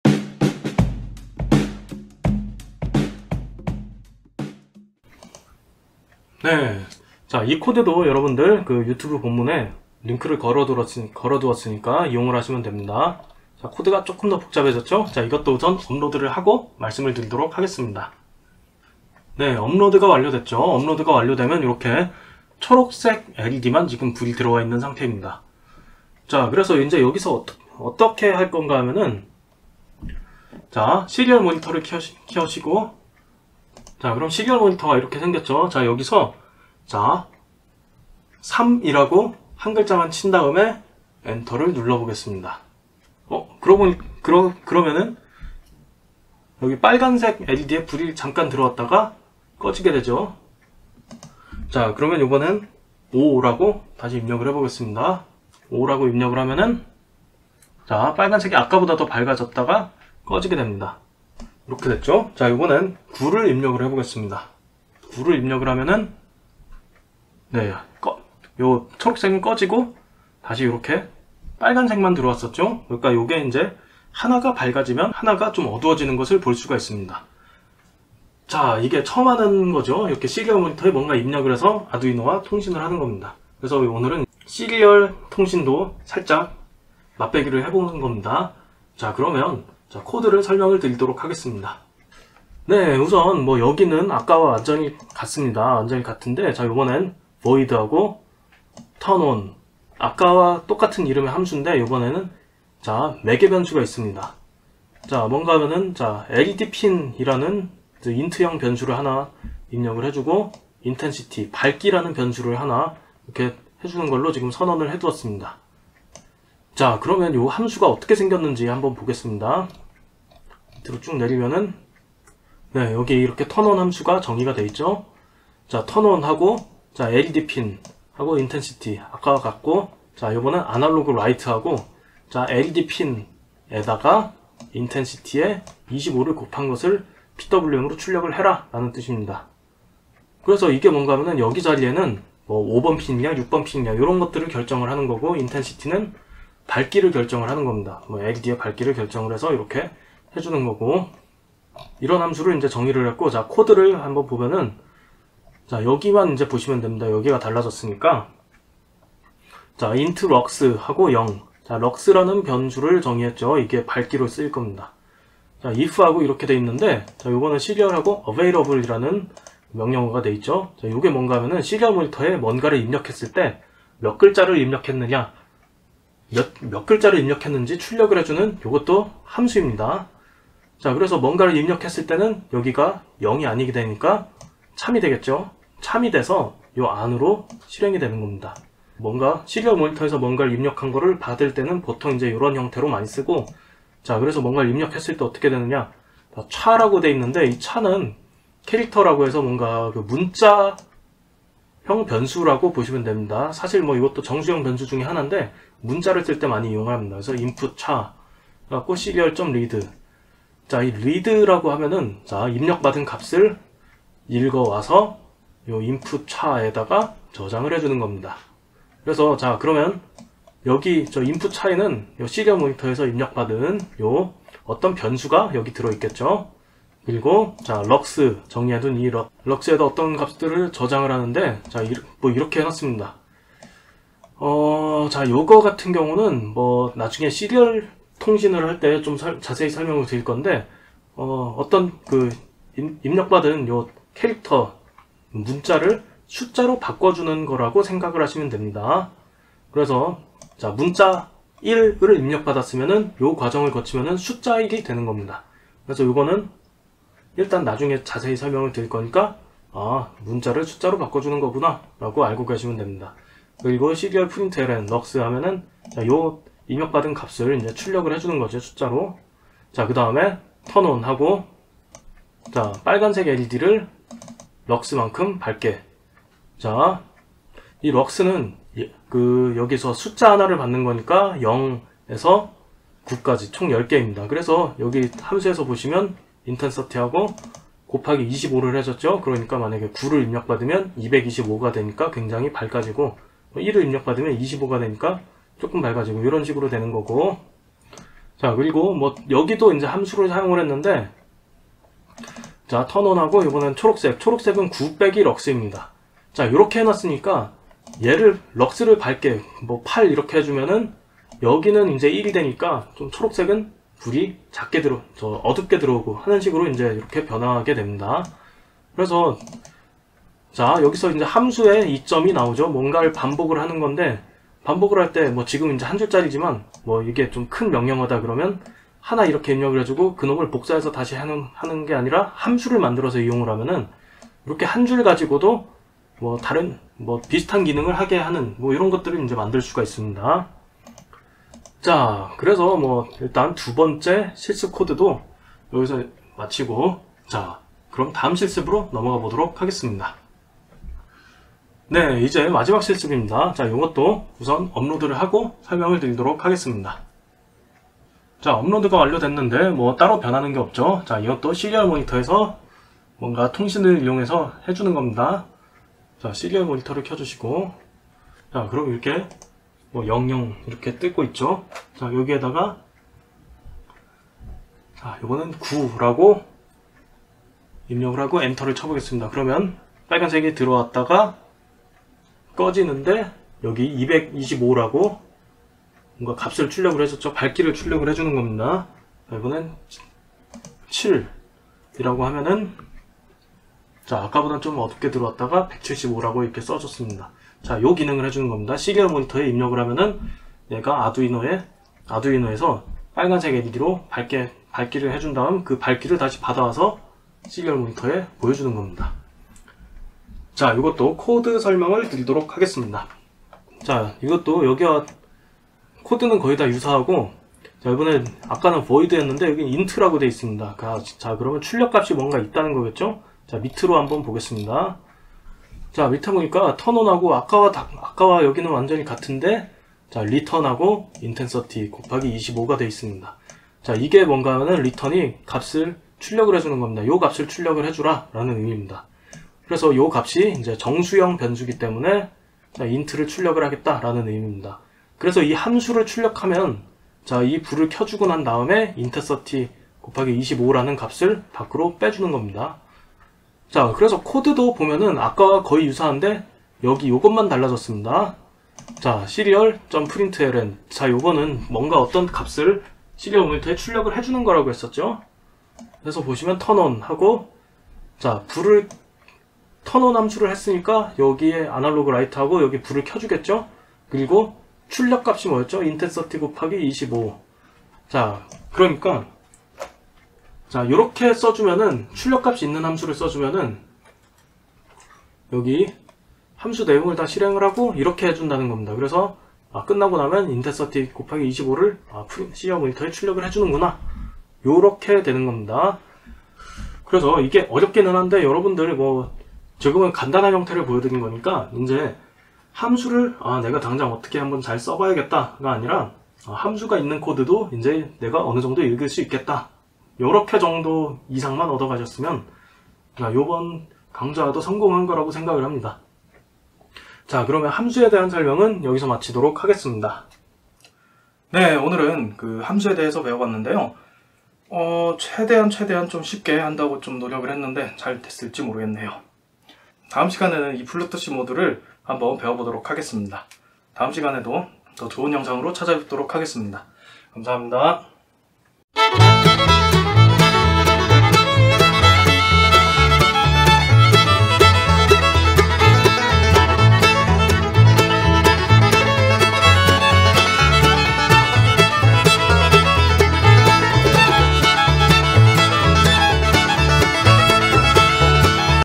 아두이노 코딩 시즌2 #4-2 || 함수 다루기 2편
00:07 실습# 4-2
05:51 실습# 4-3

안녕하세요!

공대선배입니다.

이번시간 저번영상에 이어서 함수에 대해 배워보겠습니다. 함수는 대규모의 코딩을 할때 필수적으로 사용하는 요소입니다.

아두이노의 명령어들을 하나로 묶어서 편리하게 사용하는 것이라 보면 됩니다.

최대한 자세하게 설명드려야 해서 두편으로 나누게 되었습니다.

앞으로도 열심히 하겠습니다. 감사합니다!